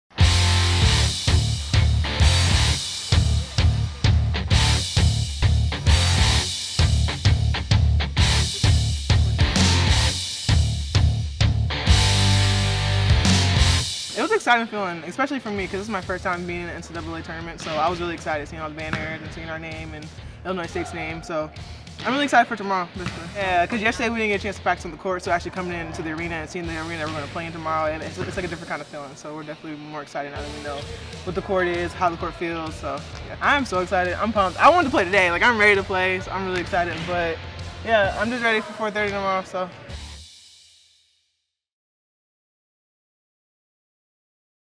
2018 NCAA Volleyball Tournament: Bearcats' Day 2 In Milwaukee
The Bearcats' head to Marquette on day 2 in Milwaukee for their first official practice before the first round of the NCAA Tournament.